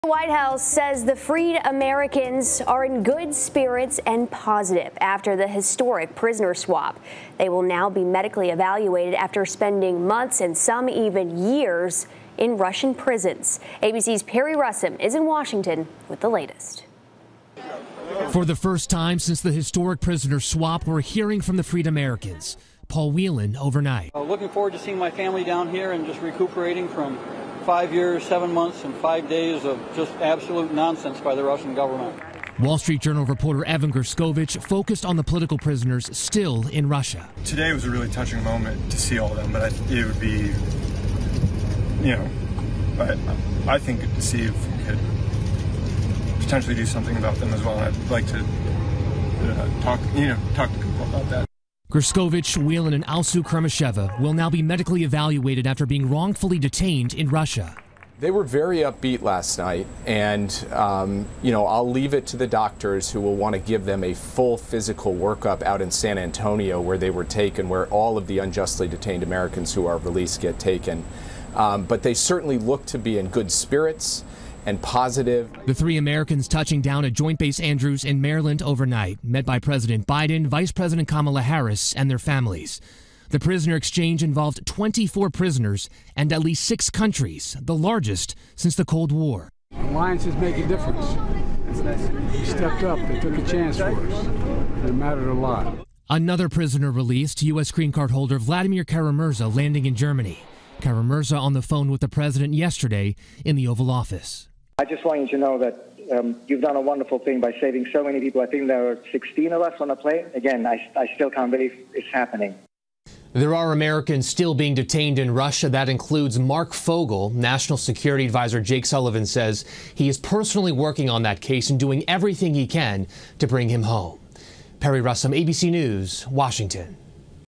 Freed Americans share experience after historic prisoner swap with Russia
The White House says that the freed Americans are in good spirits after the historic prisoner swap with Russia. Their next step is to be medically evaluated after spending months, some even years, in Russian prisons.